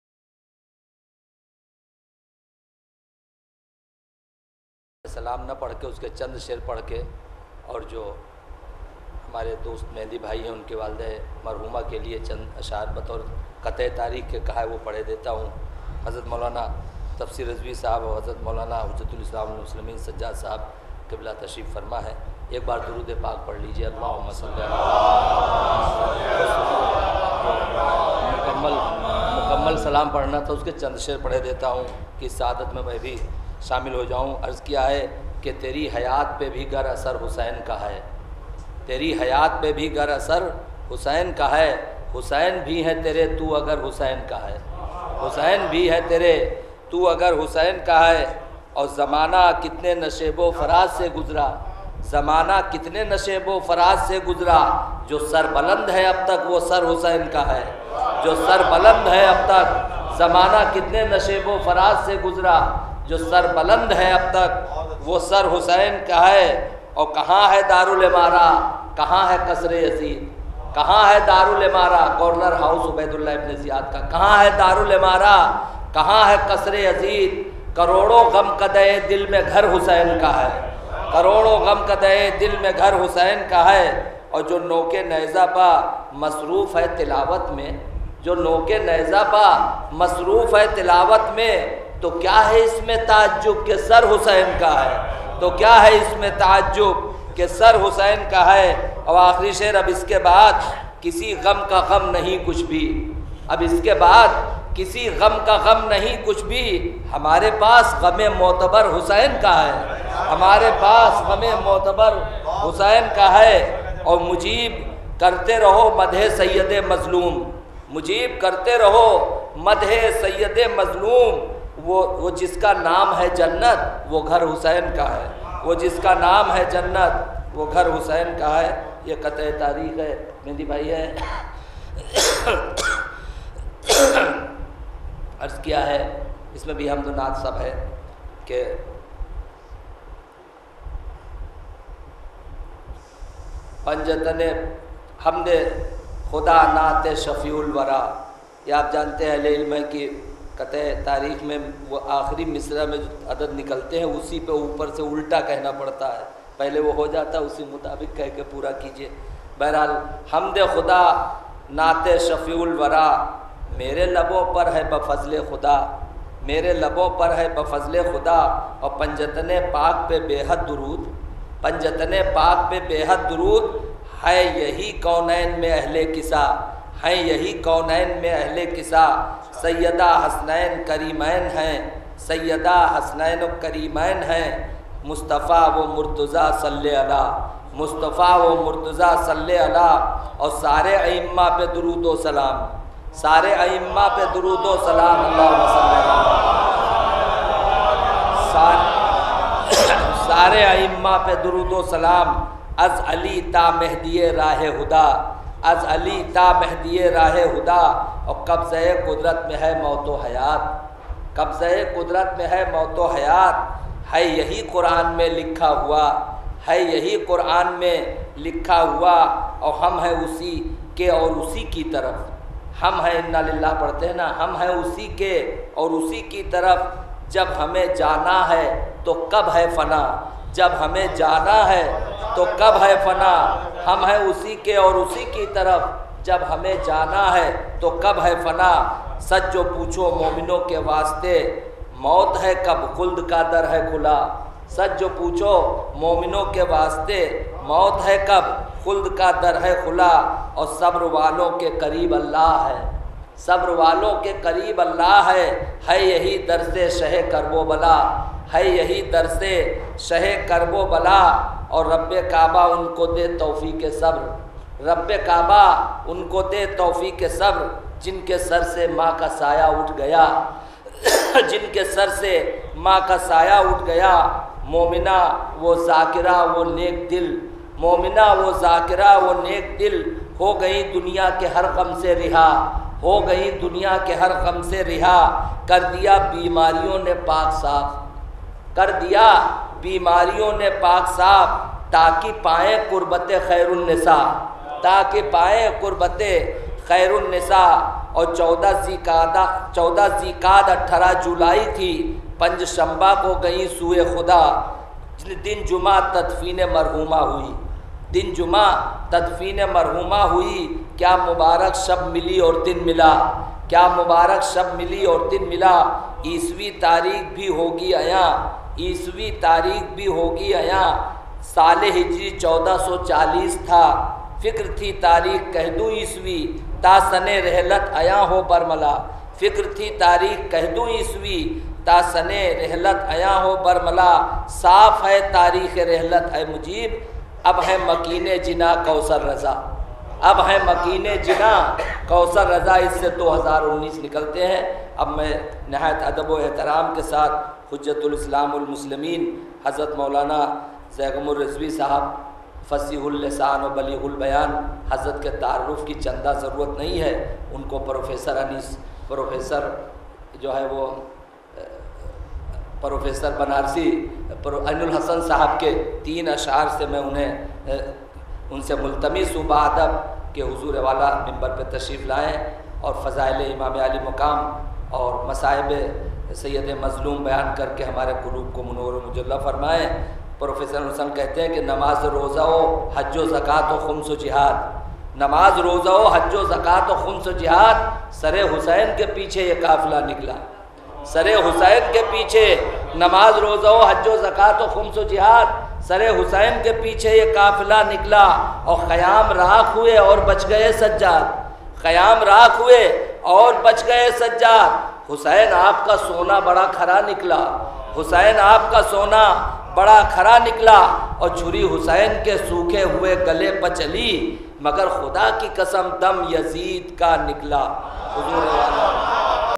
Janab Mujeeb Siddiqui | Majlis e Chehllum | Kausar Raza Binte Alamdar Raza | Gorakhpur
MAJLIS E CHEHLLUM
Bara-e-Isal-e Sawab
Mohtarma KAUSAR RAZA MARHUMA BINTE ALAMDAR RAZA MARHUM
Ahliya Mirza Iftekhar Husain
On : 24th & 25th AUGUST 2019

Our Branch Office :
GRAFH AGENCY

Presented By : Mohd. Asghar Kintoori & Karrar Haider Jaan Jarwali
Cameraman : Rahul Singh Chauhan, Mehdi Haider
Camera Asstt. : Mehdi Hasan "Sonu", Shauzab Zaidi, Meesam
Production Manager : Mahtab Husain & Mahmood Rizvi
Graphic Creation : Murtaza Bhurani
Video Editor : M. Abuzer Kintoori